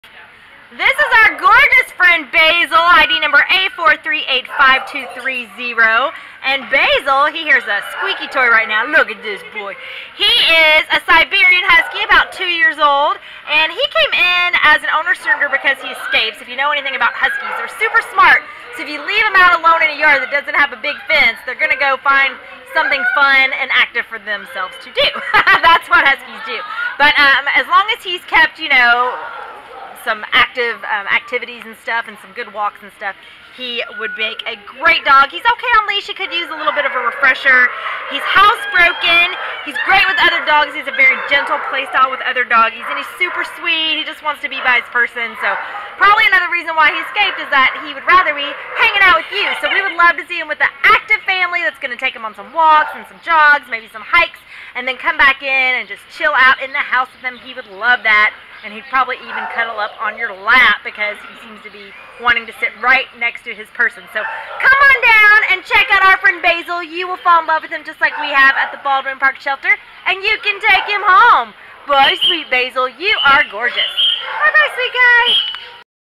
A4385230 Bayzel | Siberian Husky
A4385230 Bayzel is a magical 2-year-old black-and-white neutered male Siberian Husky whose former owners surrendered him to the Baldwin Park Animal Care Center on January 3rd because they were unable to contain him. Weighing 50 lbs, Bayzel is an affectionate and easy-going pooch who enjoys being with people and getting pet. He was happy to plop into our volunteer’s lap when she sat down on the ground next to him. Bayzel has a relaxed energy level, but like all dogs--and especially Huskies--he will need regular exercise and stimulation to keep him happy. We noticed he has a keen interest in squeaky toys and we bet he would enjoy puzzle balls too. Bayzel has been calm in the shelter’s playgroups, and not terribly interested in playing with other dogs, so we expect he would be happiest as an only dog or with another laid-back dog.  Bayzel walks fine on leash, but may benefit from some additional leash training to be a perfect walking companion. Bayzel will be a magnificent best friend in an active and fun-loving household eager to make a smart, attentive dog a big part of the family.